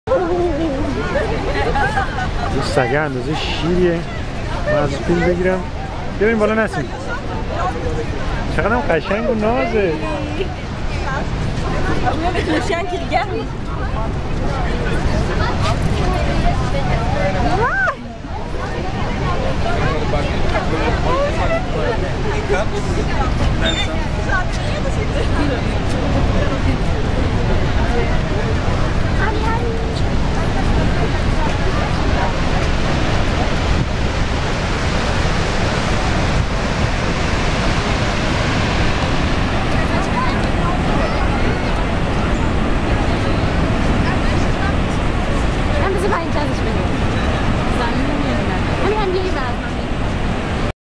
NiagaraFalls-Canadian Dog.MOV
We were in Niagara Falls at Aug 6 2010, and saw this amazing dog. The owner told me that  the dog is from Newfoundland and all dogs in this race are this big and cute. The most beautiful and elegant dog ever !!!